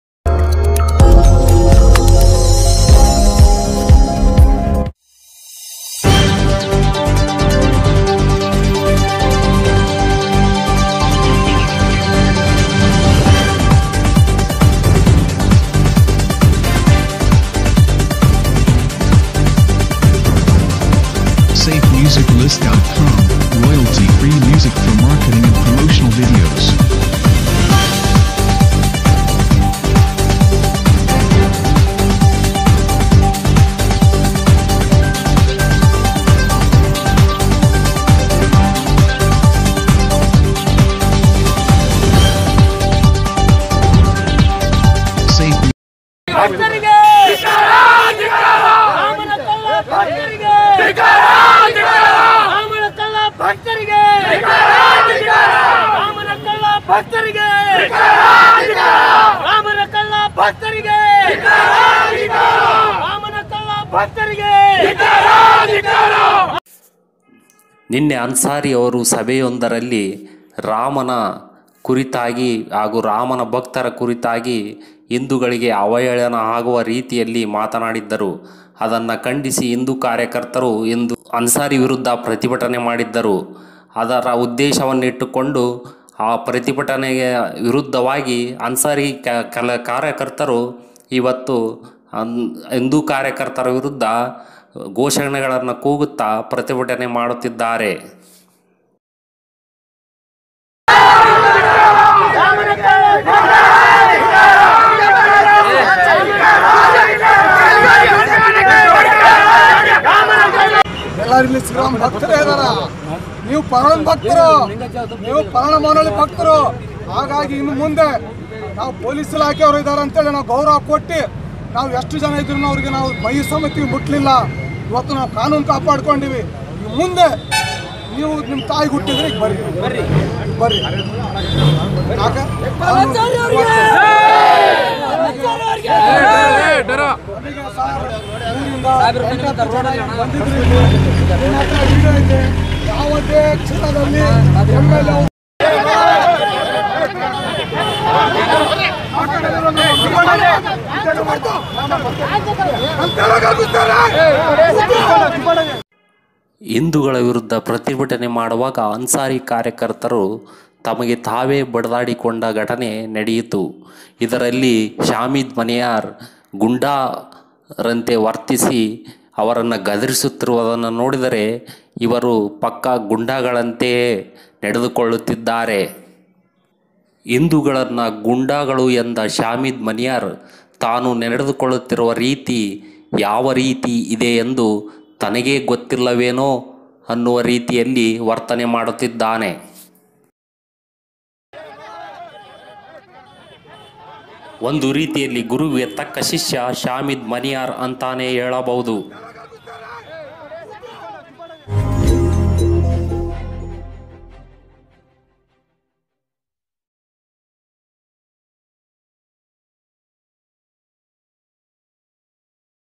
ಹಿಂದೂ ಕಾರ್ಯಕರ್ತರ ವಿರುದ್ದ ಪ್ರತಿಭಟನೆ ಮಾಡುವಾಗ ತಮಗೆ ತಾವೇ ಬಡಿದಾಡಿಕೊಂಡ ಅನ್ಸಾರಿ ಕಾರ್ಯಕರ್ತರು
Ansari activists who have been stabbed by themselves while protesting against Hindu activists

koppal district news, gbnews kannada